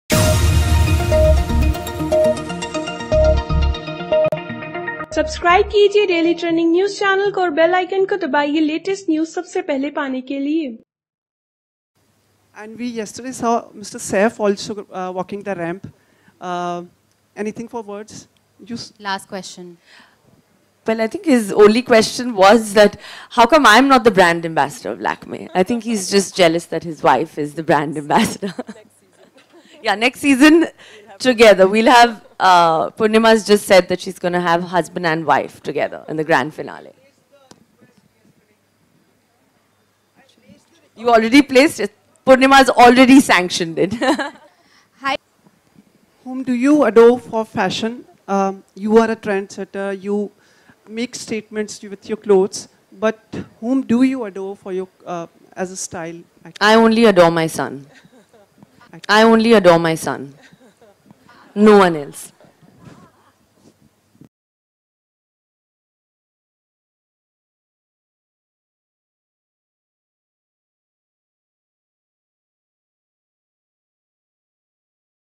Kareena Kapoor's SHOCKING Comment On Personal Life With Saif Ali Khan In Public
Edited By -- Paddy